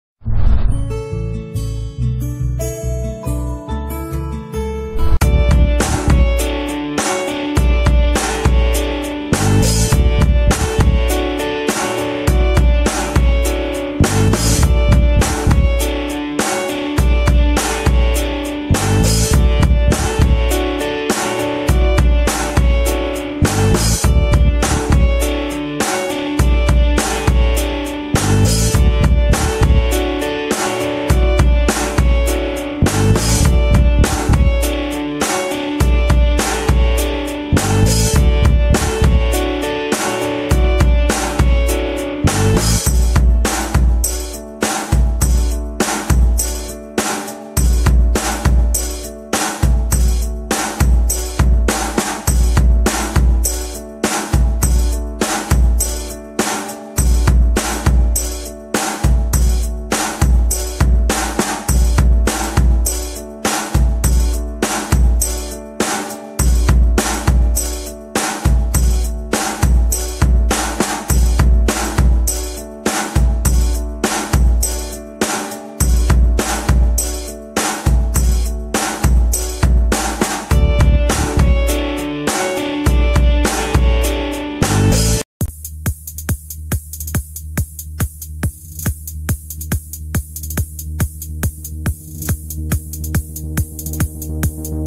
[Breaking News]Manchester United transfer news LIVE
The mood is positive in the Manchester United camp. Jose Mourinho's side beat Yeovil 4-0 to go throu...